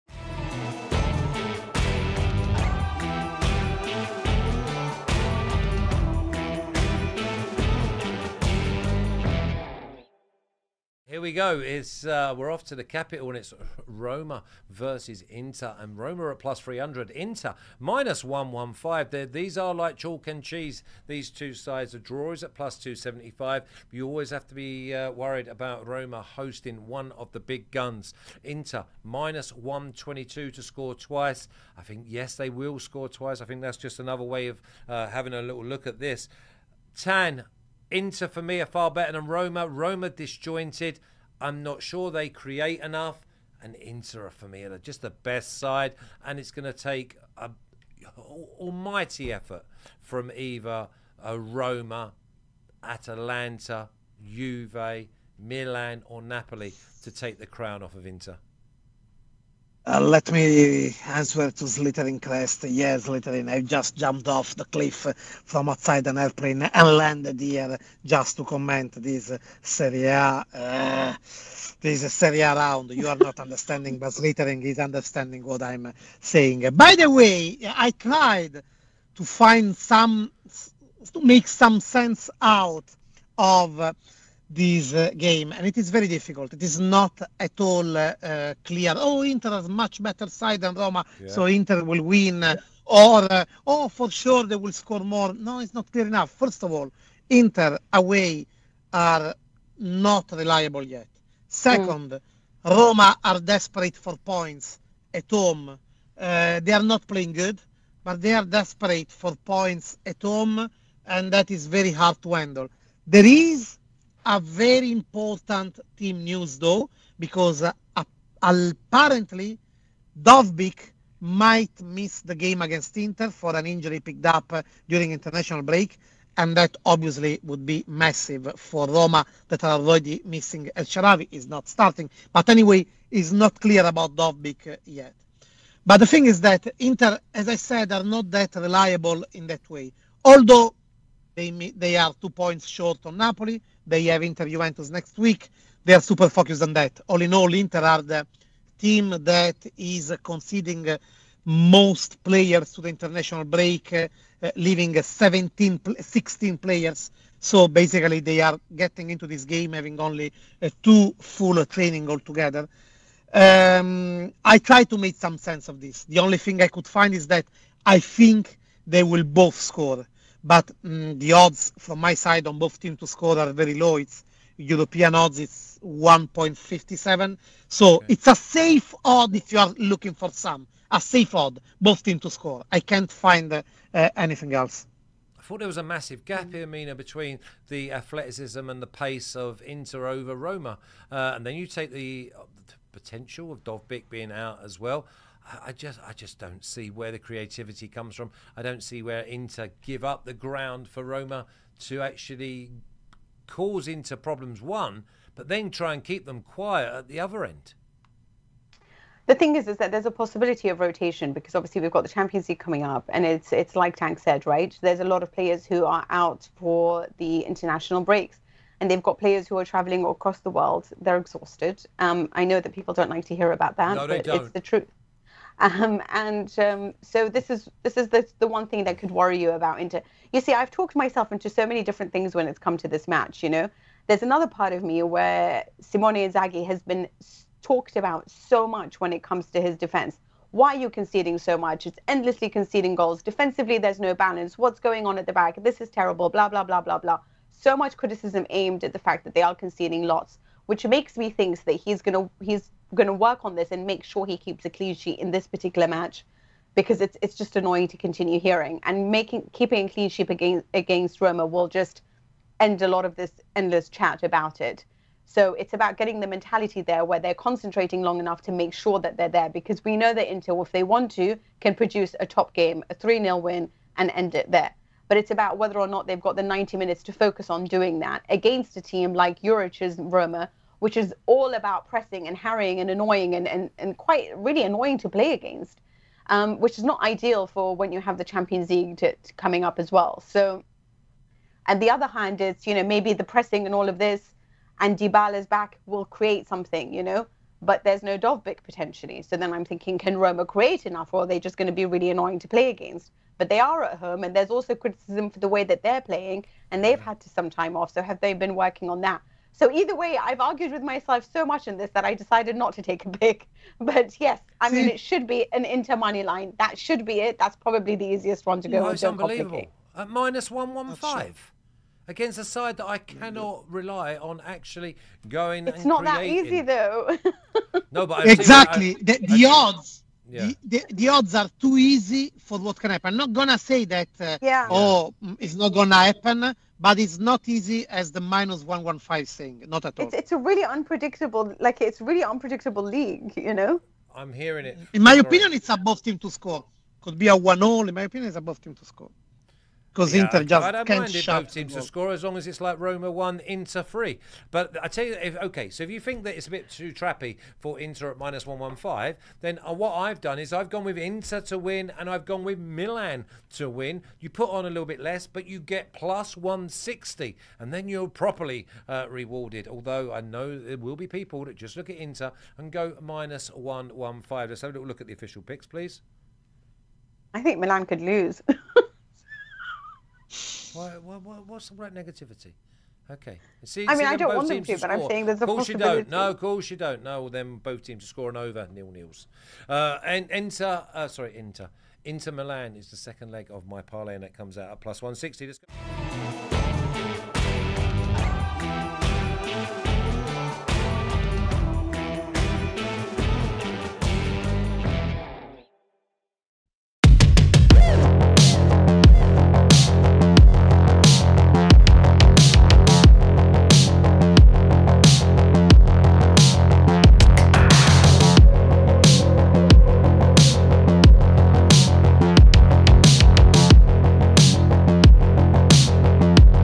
Roma vs Inter | Serie A Expert Predictions, Soccer Picks & Best Bets
Roma vs Inter Serie A game preview for October 20th, 2024

Our betting experts preview the Serie A game between Roma vs Inter. They'll share their soccer picks, Serie A expert predictions, the latest sport betting trends, and best value soccer odds available for their betting predictions, soccer picks and best bets.

Connect with our hosts and experts on X:
Flash: @Flash_37
Tancredi: @tancredipalmeri
Mina: @Minarzouki